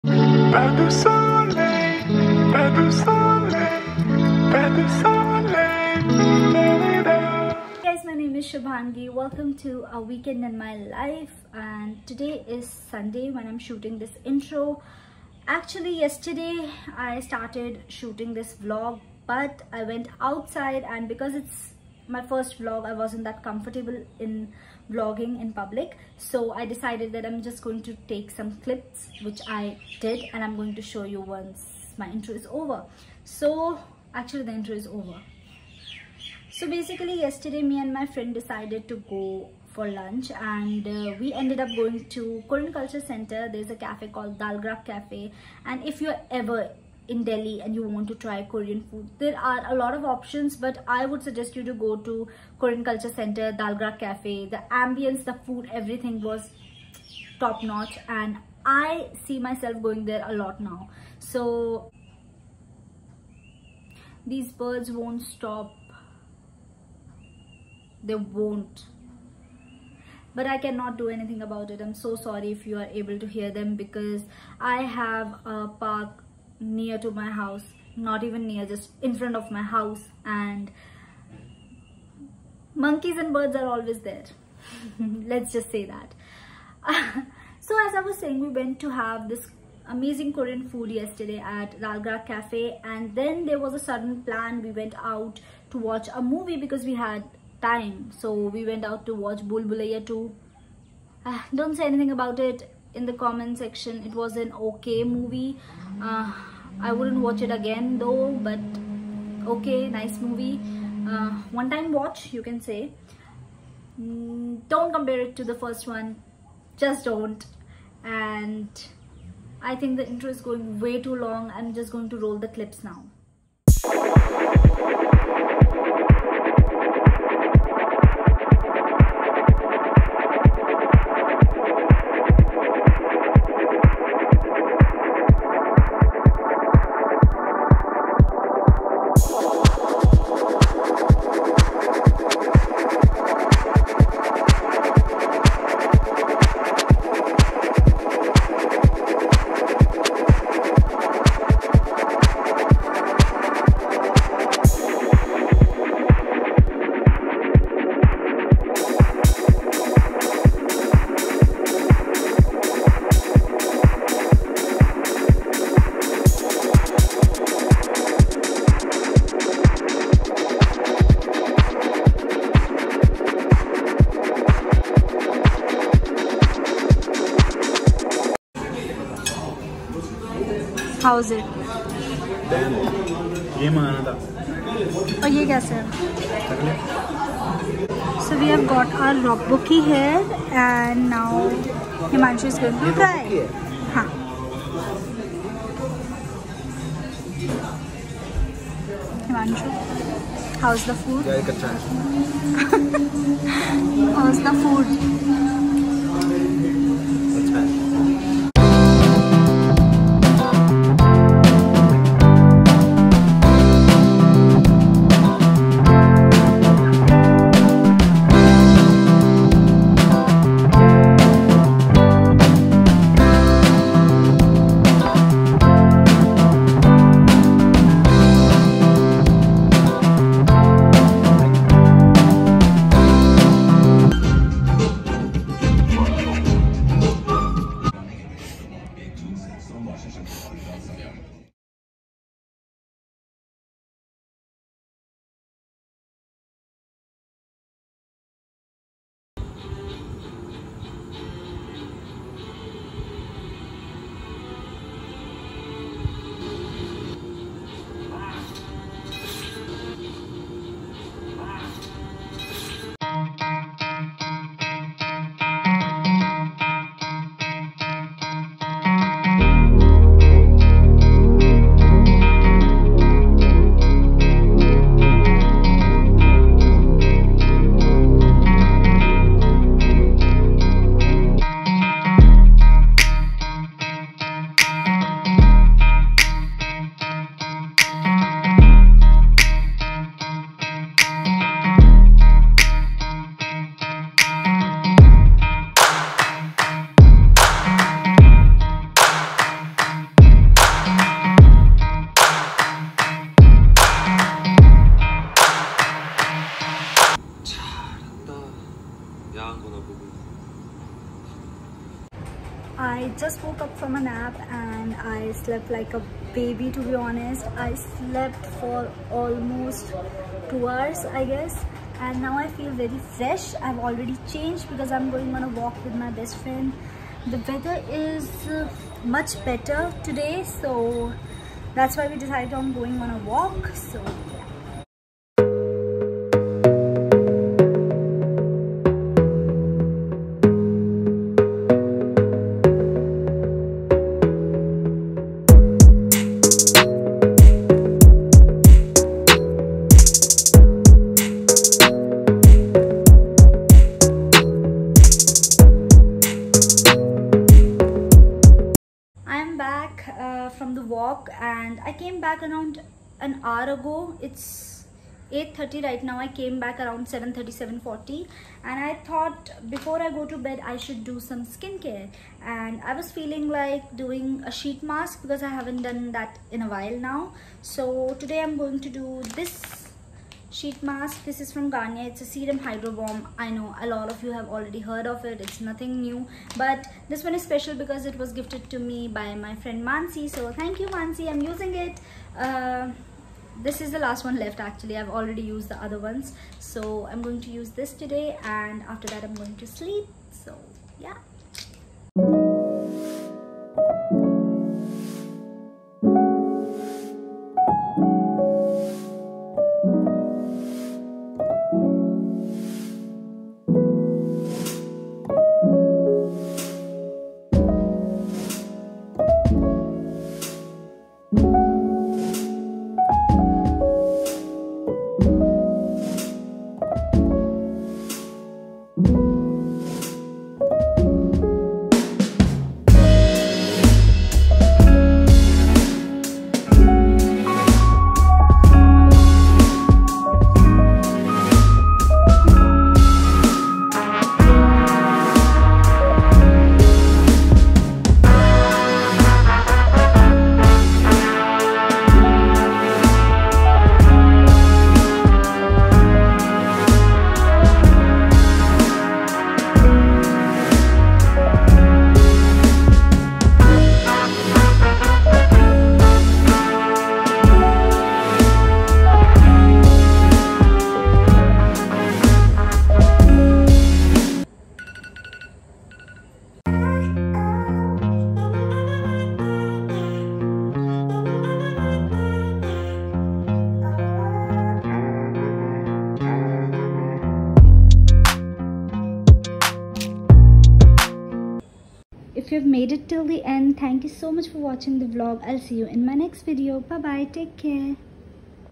WEEKEND in my life | trying korean food | vlog | SHUBH
Hey Stars! This is weekend in my life. From watching Bhool Bhulaiyaa 2 to trying Korean food at Korean Cultural Centre (Dalgrak cafe), I tried to vlog everything I did past weekend.